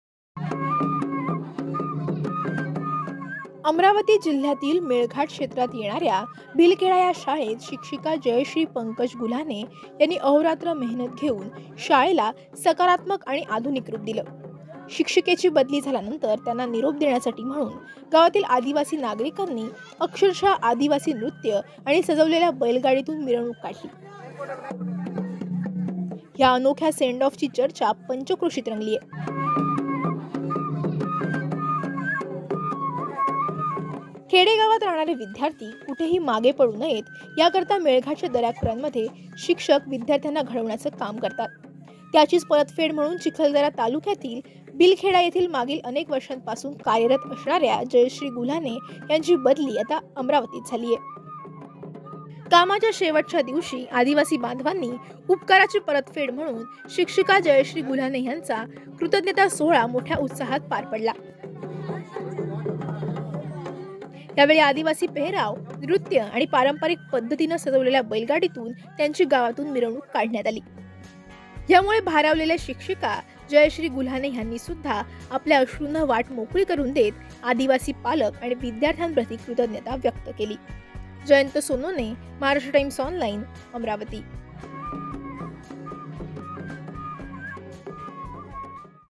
Emotional Farewell To Teacher : आदिवासी नृत्य, सजवलेल्या बैलगाडीतून मिरवणूक; शिक्षिकेला अनोखा निरोप
अमरावती जिल्ह्यातील मेळघाट क्षेत्रात येणाऱ्या भिलखेडा या शाळेत शिक्षिका जयश्री पंकज गुल्हाने यांनी अहोरात्र मेहनत घेऊन शाळेला सकारात्मक आणि आधुनिक रूप दिले. शिक्षिकेची बदली झाल्यानंतर त्यांना निरोप देण्यासाठी म्हणून गावातील आदिवासी नागरिकांनी त्यांची आदिवासी नृत्य करत सजवलेल्या बैलगाडीतून मिरवणूक मिरवणूक काढली. या अनोख्या सेंड ऑफची चर्चा सध्या पंचक्रोशीत रंगली आहे.